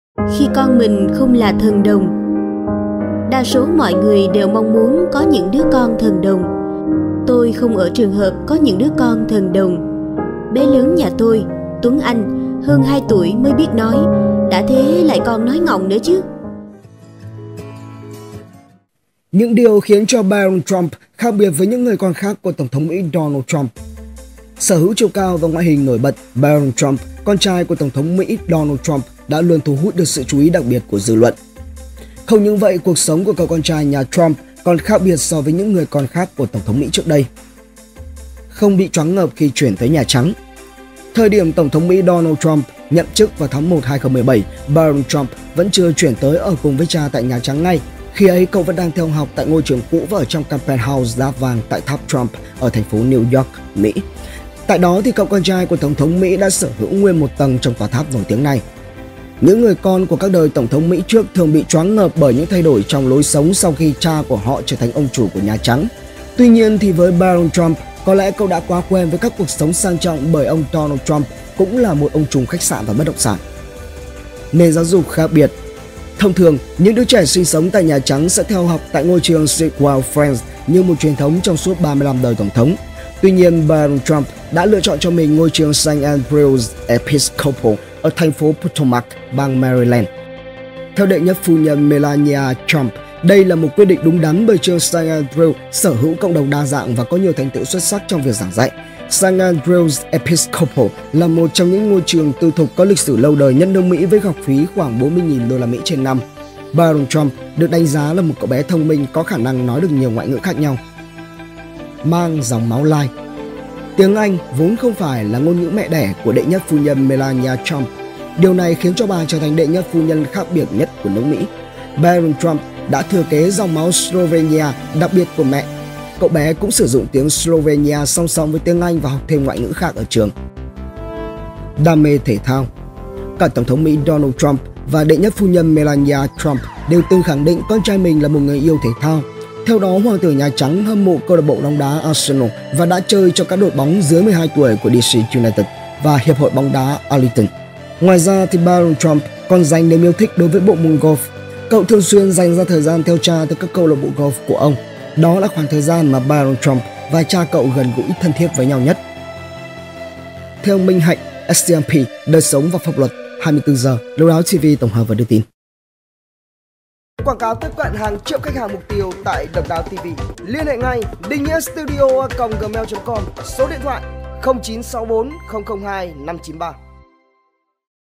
Barron Trump & Những Điều Khác Biệt Với Những Người Con Khác Của Các Tổng Thống Mỹ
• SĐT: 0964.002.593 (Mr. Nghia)

Sở hữu chiều cao và ngoại hình nổi bật, Barron Trump, con trai Tổng thống Mỹ Donald Trump, đã luôn thu hút sự chú ý đặc biệt của dư luận. Không những vậy, cuộc sống của cậu con trai nhà Trump còn khác biệt so với những người con của các Tổng thống Mỹ trước đây.